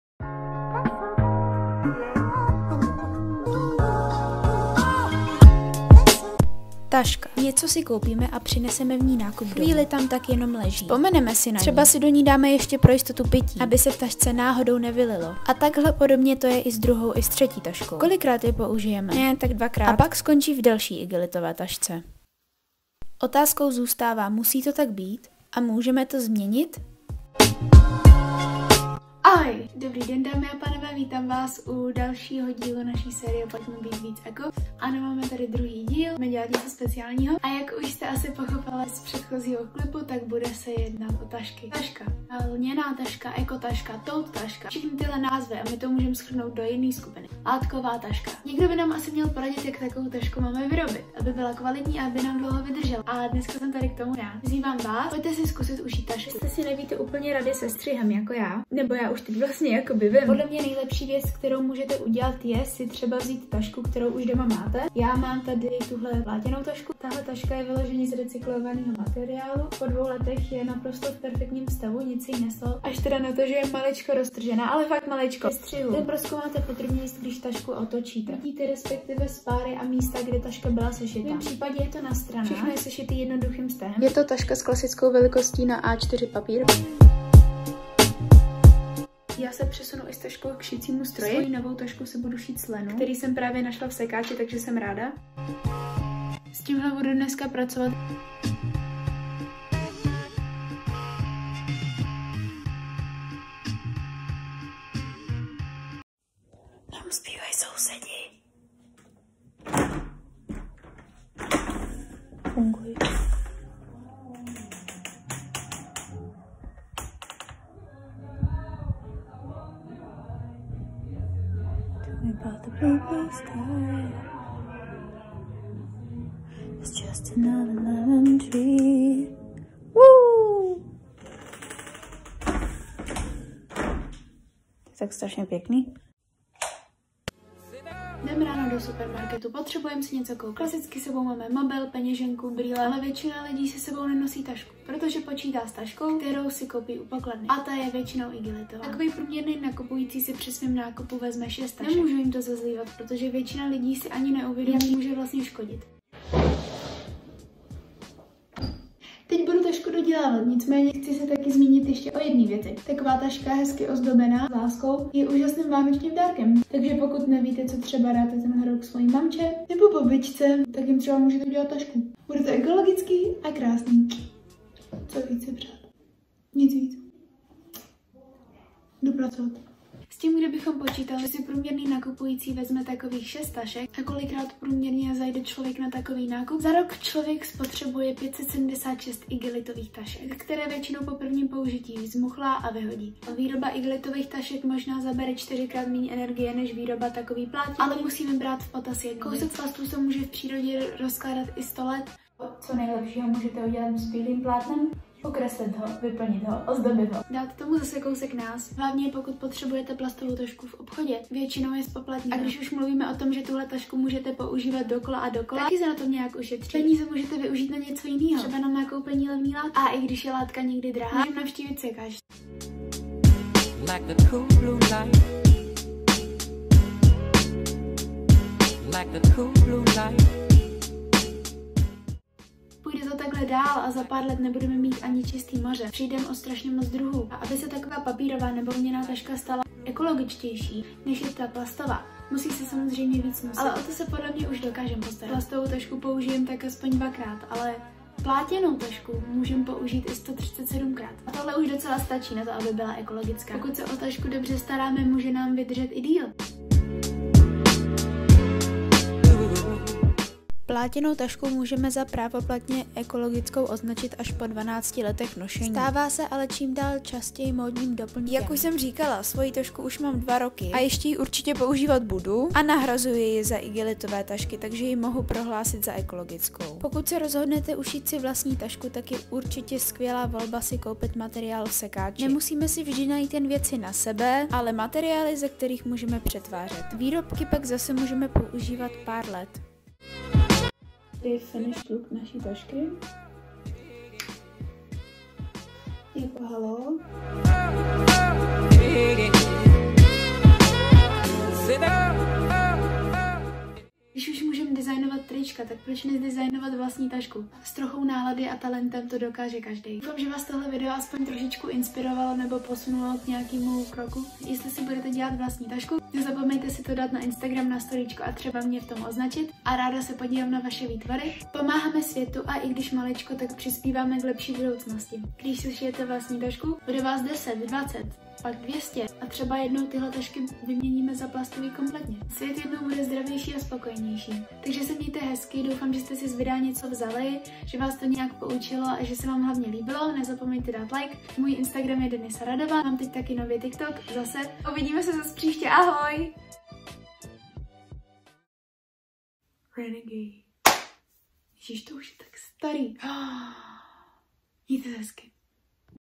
🦋DIY EKO TAŠKA✨, šití, zdobení | Pojďme být víc eko
Ahojda!🦋

V dnešním videu budu šít a zdobit svojí novou plátěnou tašku! Najdete zde také nějaké eko tipy jak šetřit a snad se naučíte i něco nového.

~Kde ještě jsem~
TikTok ~ @denisaradova

Určitě napište a šijte tašky!!

Papaaa;))